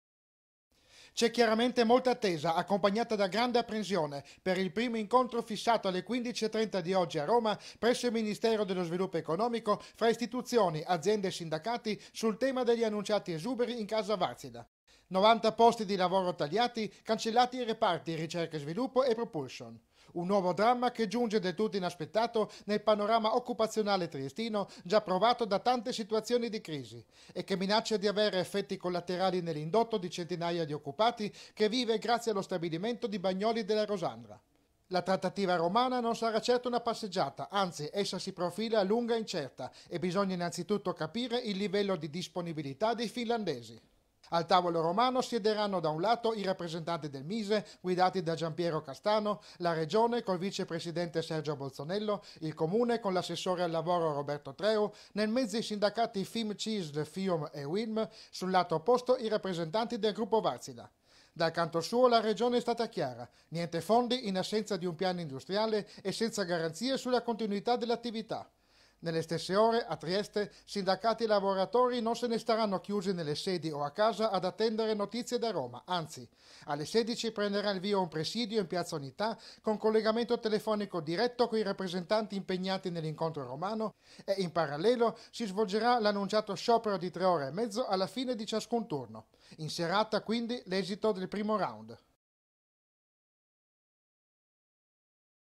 12/05/2016 - WARTSILA, LA CRISI SUL TAVOLO DEL MINISTERO DELLO SVILUPPO ECONOMICO
IL NOTIZIARIO (giovedì 12 maggio 2016) - Si apre oggi a Roma presso il Ministero dello sviluppo economico il tavolo di confronto con la Wartsila che ha annunciato 550 esuberi di cui 90 riguardano lo stabilimento triestino di Bagnoli della Rosandra